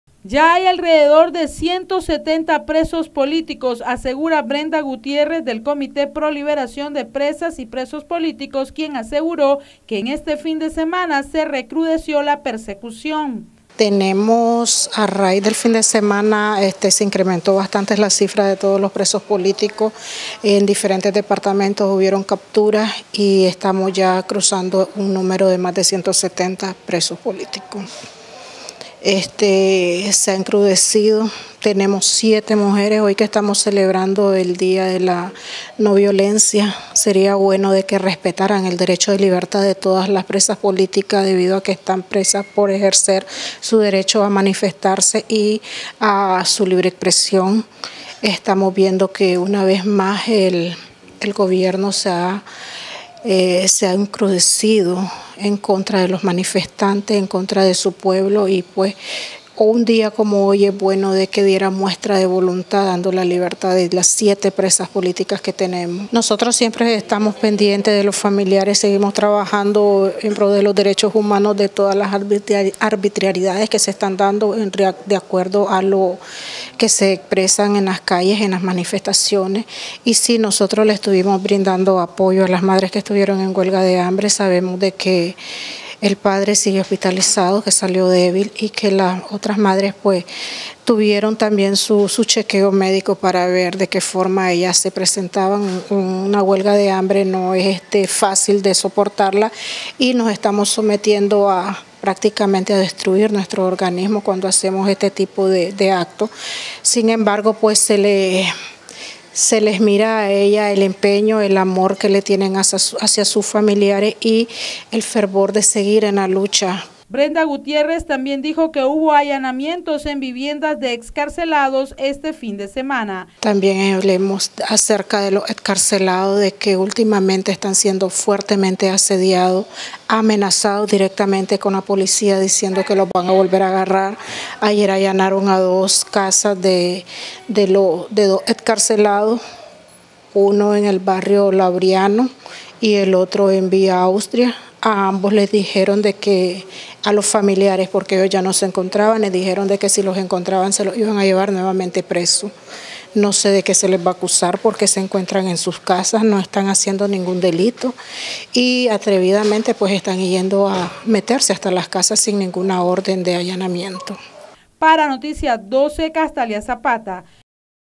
¨PRESOS POLÍTICOS ASCIENDEN A LOS 170¨
Los presos políticos ya ascienden a los 170, según Brenda Gutiérrez, del comité de pro liberación de presas y presos en el contexto de protestas.